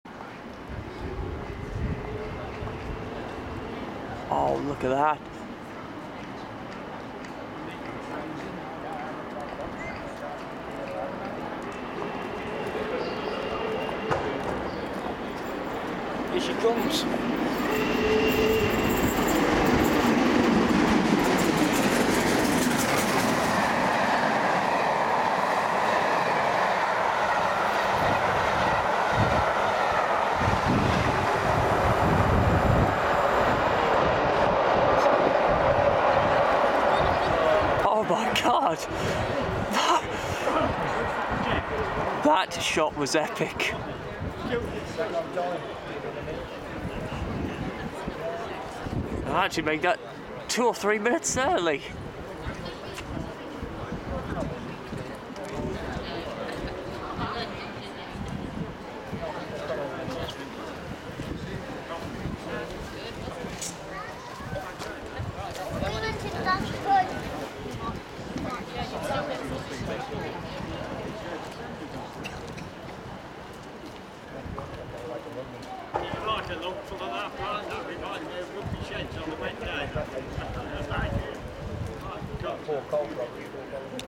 6201 Princess Elizabeth blasting through Coventry, 17th July 2010
In truly epic fashion, No.6201 'Princess Elizabeth' thunders through Coventry on July 17th 2010, leaving a trail of smoke behind as she heads towards London Euston.  My reaction on 'Lizzie's' disappearance pretty much sums it all up.